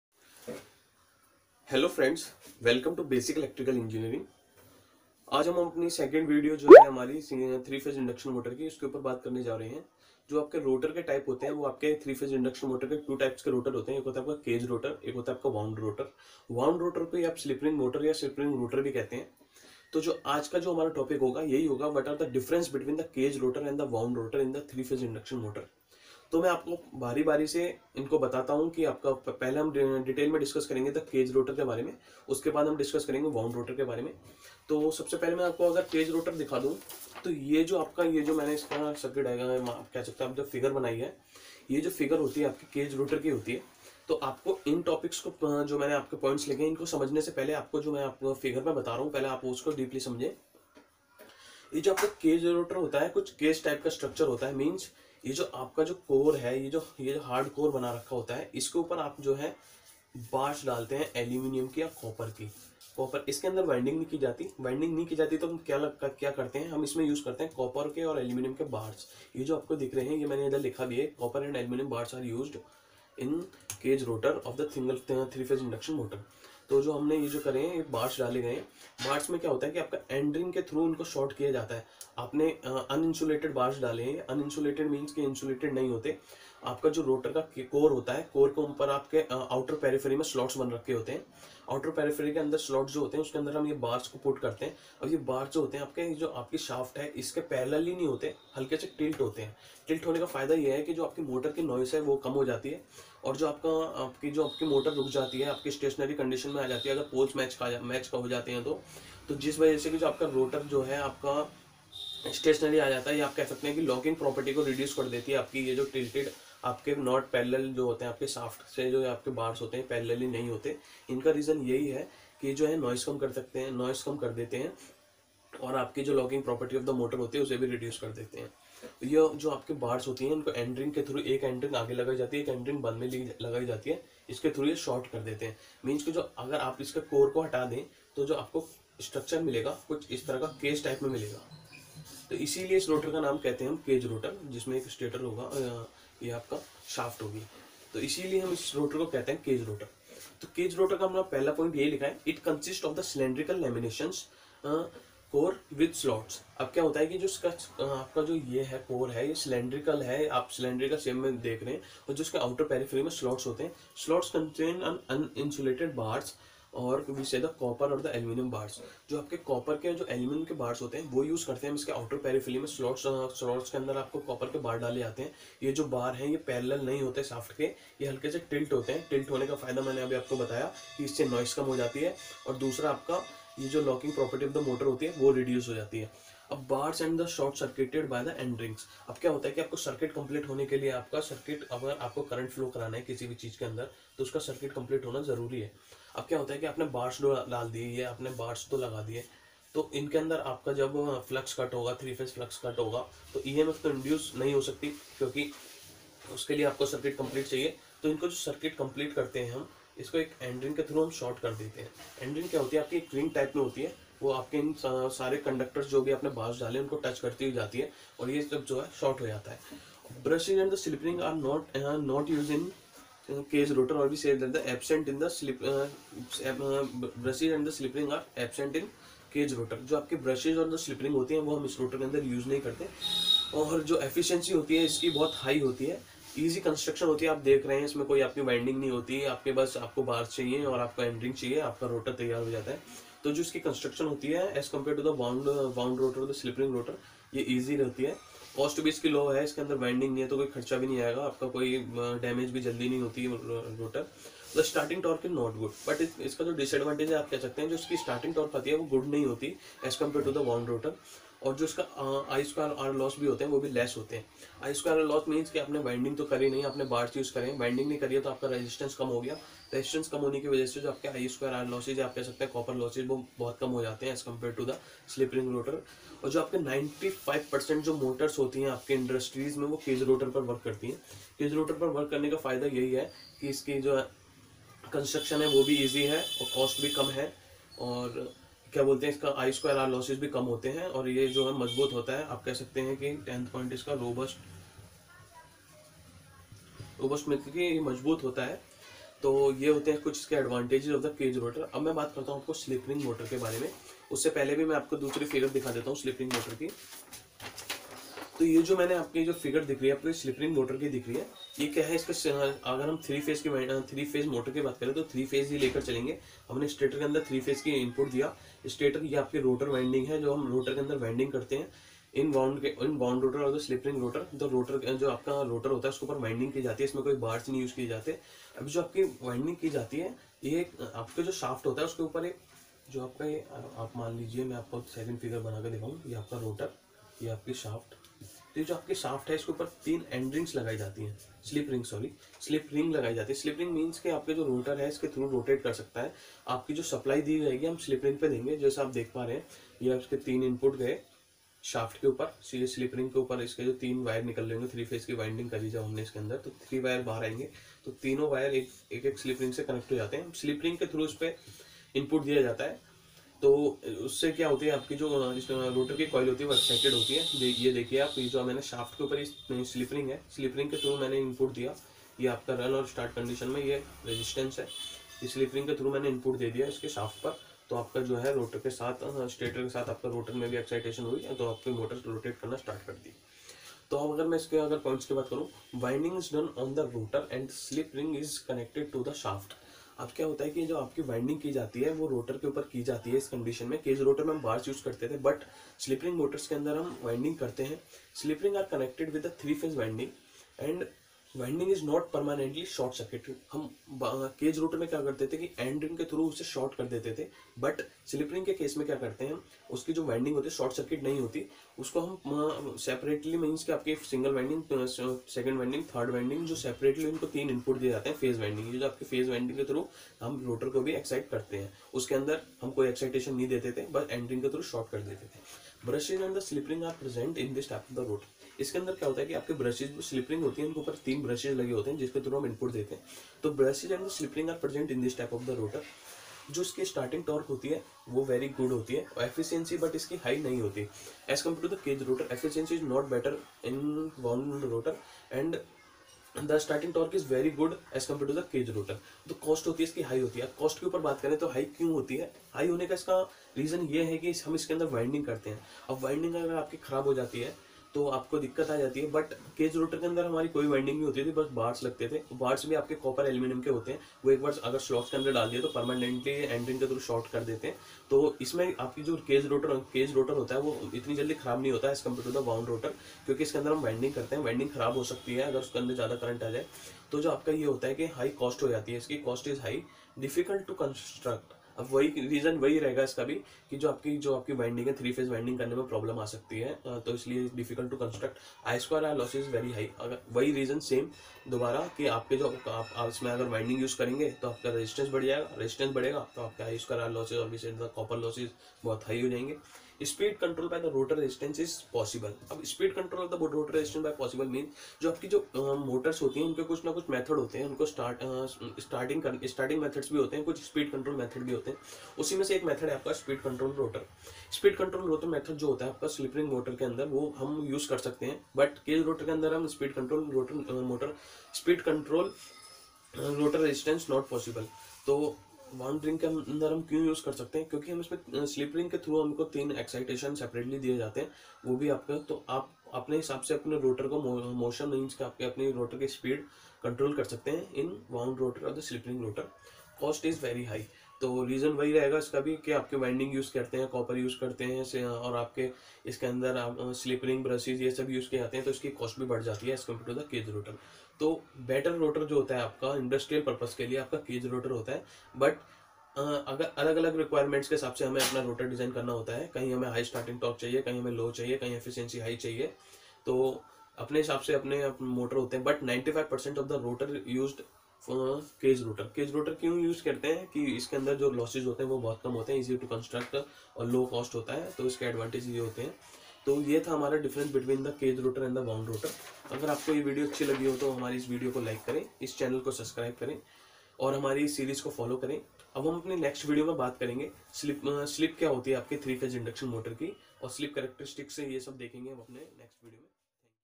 Difference Between Cage Rotor and Slip Ring Rotor |basic electrical engineering
Difference Between Cage Rotor and Slip Ring Rotor
A wound-rotor is a type of induction motor where the rotor windings are connected through slip rings to external resistance. Adjusting the resistance allows control of the speed or torque characteristic of the motor. The slip ring rotor or wound rotor has very little application in industries